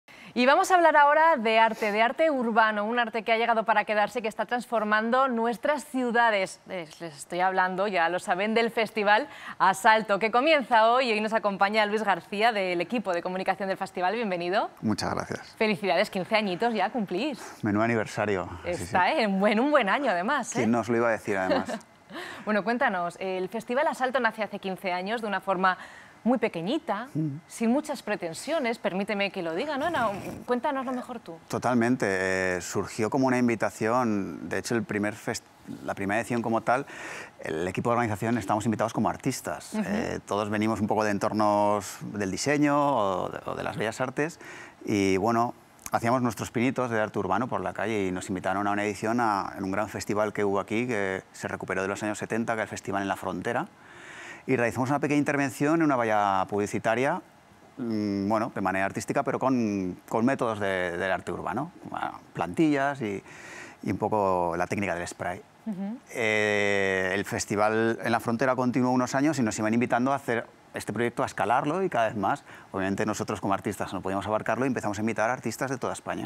Comienza el Festival Asalto cumpliendo 15 años
El festival que pone en alza el arte urbano, empieza hoy con 15 años a sus espaldas.